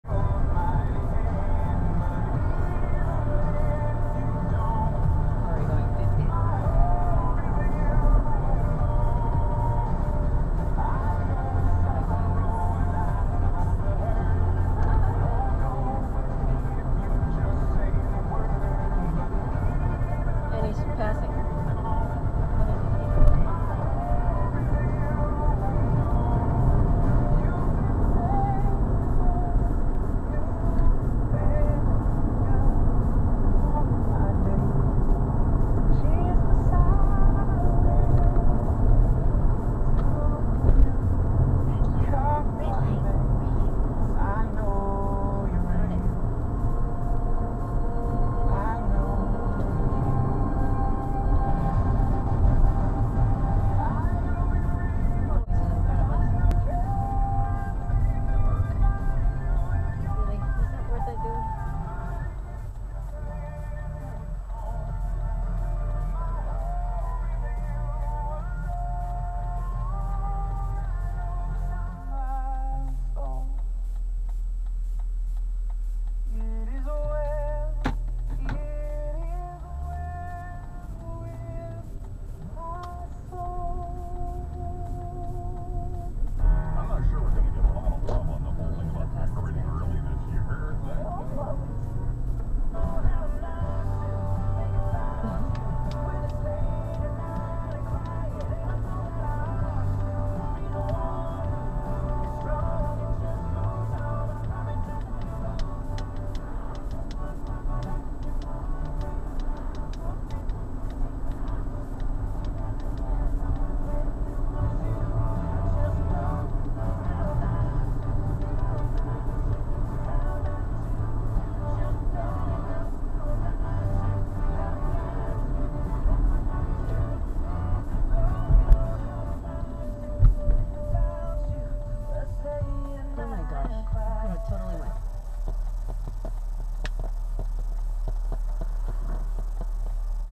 11-12-18 Driving: In a hurry . . .
Toyota Corolla, westbound on Clinton Keith Road
Some drivers just make you think: "Why do you need to drive like that"? and "How do you not get caught"? They seem to be very lucky.